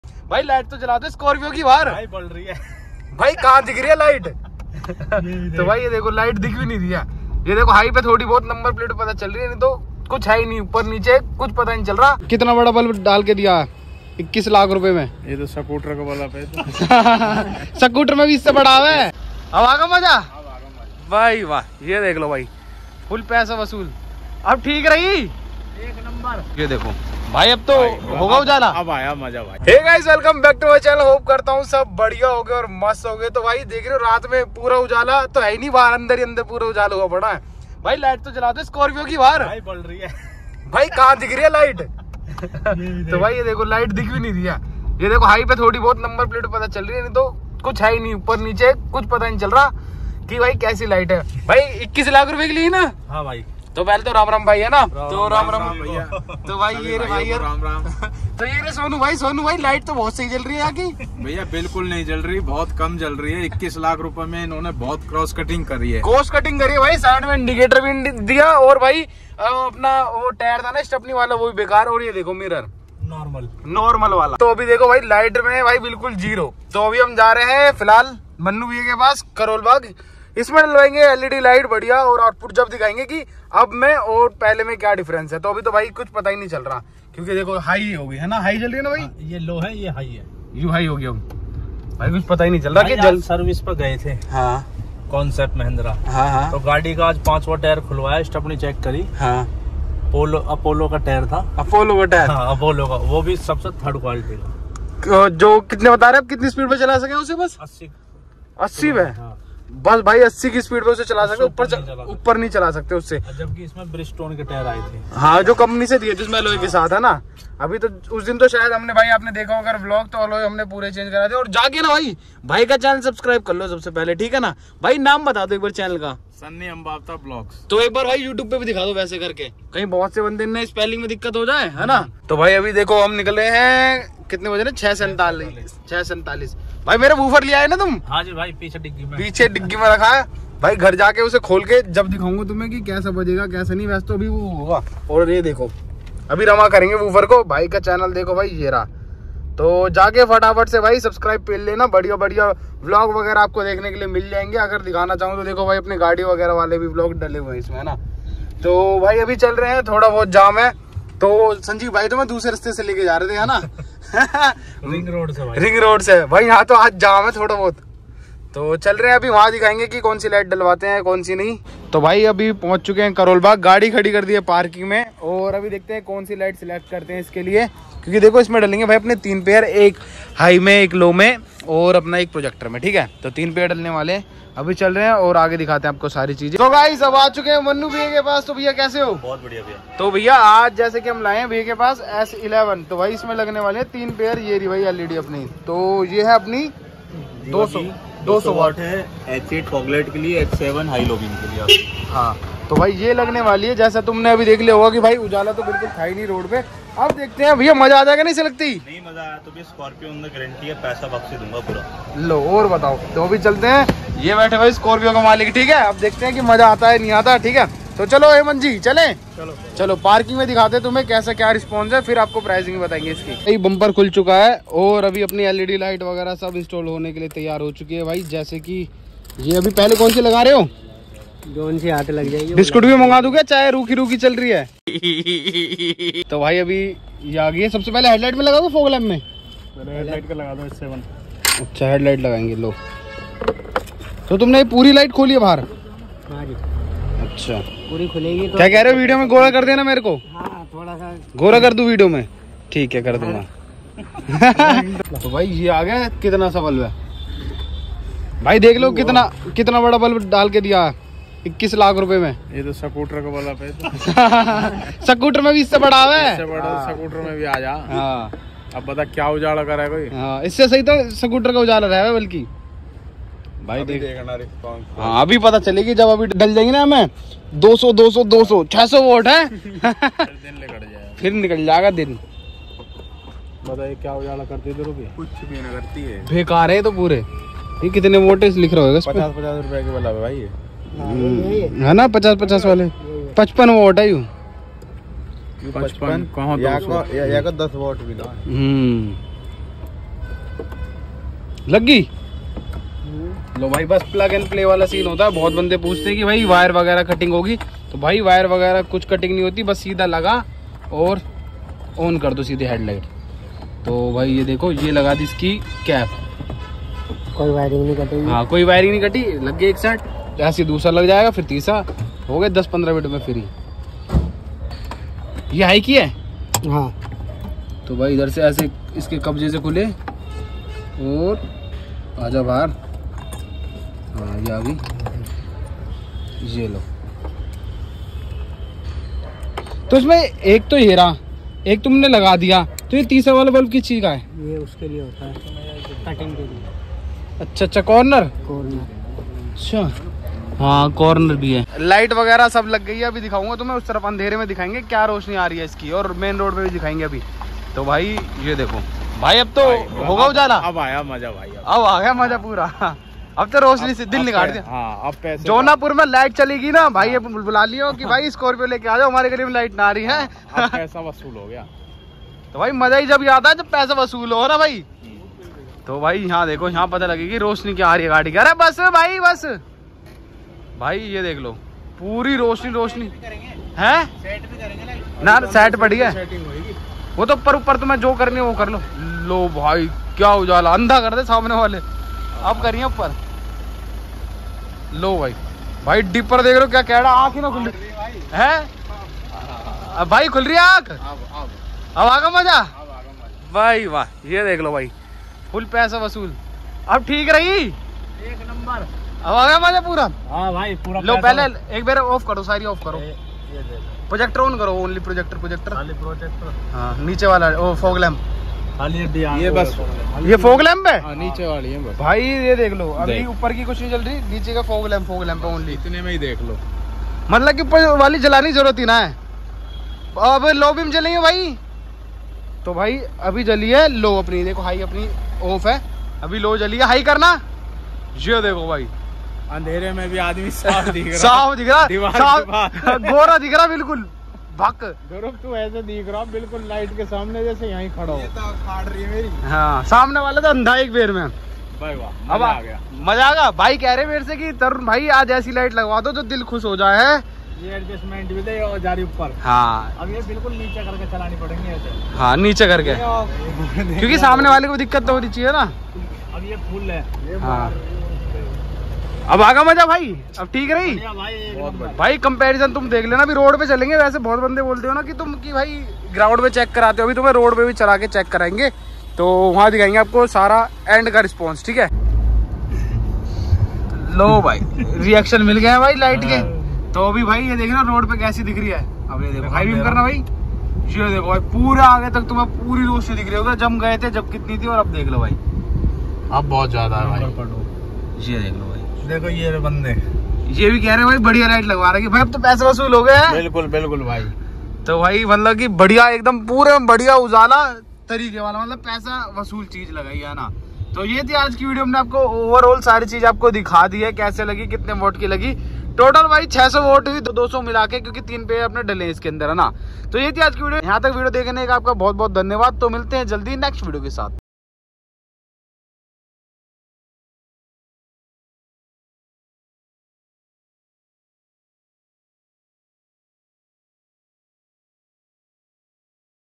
😱🔥Scorpio Classic में लगवा दी 600Watt की Led Light Best Lights For Mahindra 3 Pair led lights
Instagram Account 👇👇👇👇👇👇👇👇👇👇👇👇👇 Plz Follow

Shiva car accessories
Address  = Shiva car accessories Karol Bagh Delhi India 110005